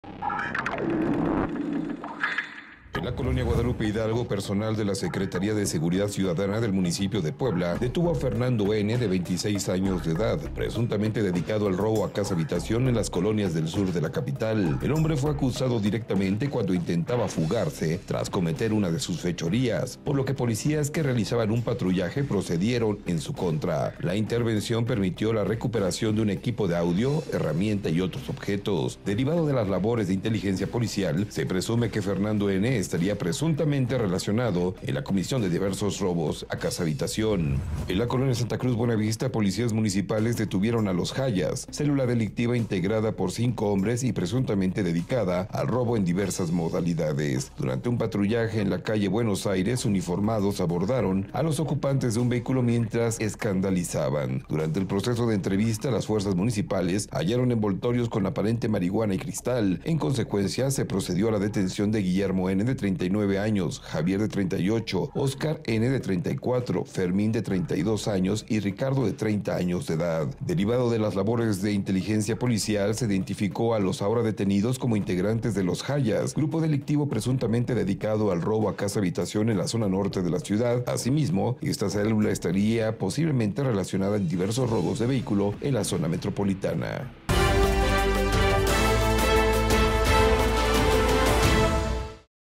Robo a casa habitación - Reporte 4.1 | Las Noticias Puebla
En la colonia Guadalupe Hidalgo personal de la Secretaría de Seguridad Ciudadana,  detuvo a Fernando "N" por el delito a casa habitación